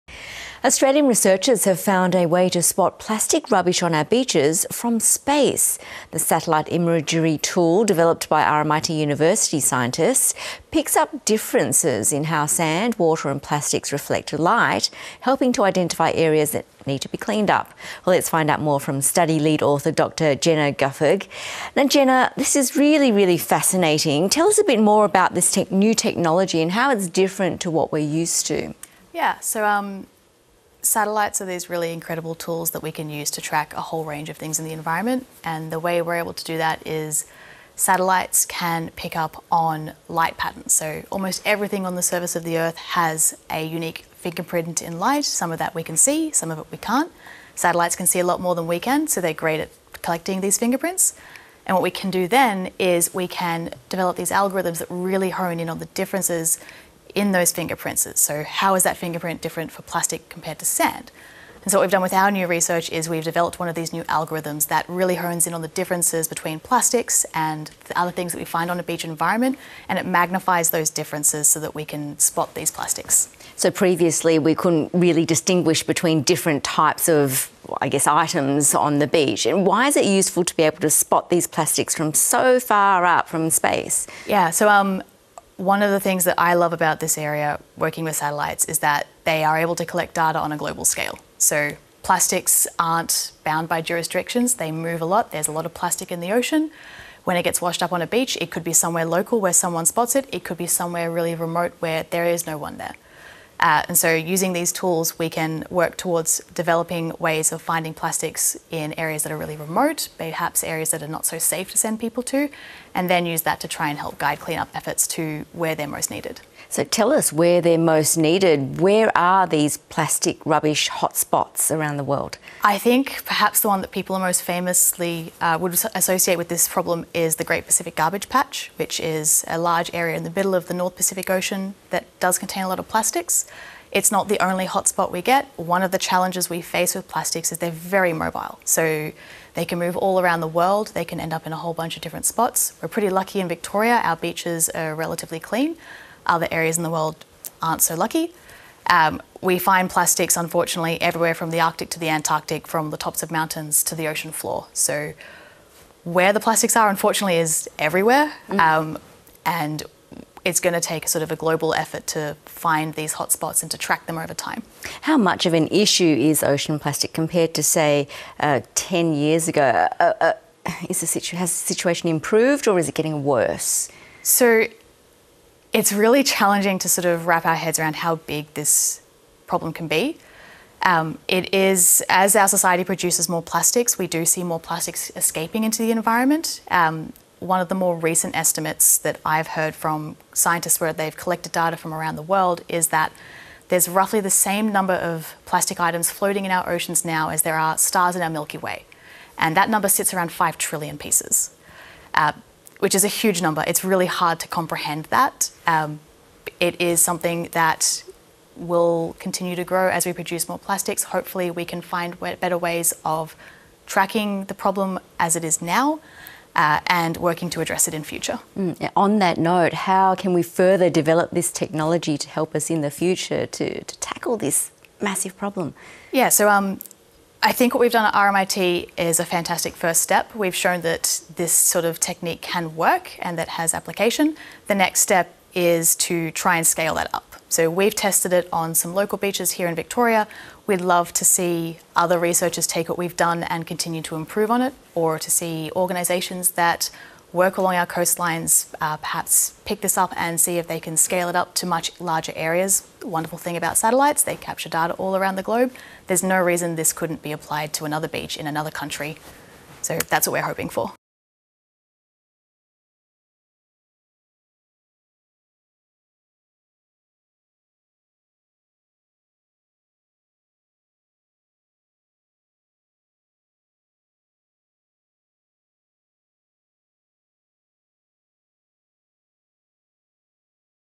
High-tech way to spot plastic beach rubbish... from space | ABC News
RMIT University scientists have found a way to spot plastic rubbish on our beaches from space, using a satellite imagery tool to pick up differences in how sand, water and plastics reflect light.  The study's lead author Dr Jenna Guffrogg tells Yvonne Yong the technology could be used to find plastic in places "that are not so safe to send people to, and then use that to try and help guide clean-up efforts to where they're most needed".